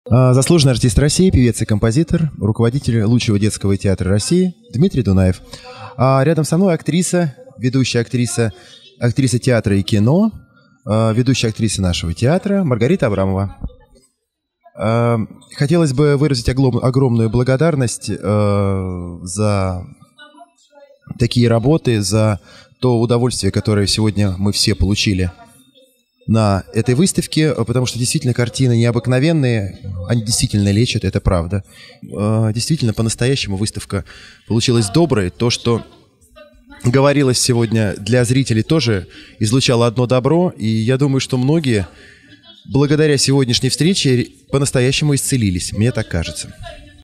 Видеоотзыв с творческого вечера В.ПреобРАженской (1)
Видеоотзывы с творческого вечера Виктории Преображенской, Москва, 2009.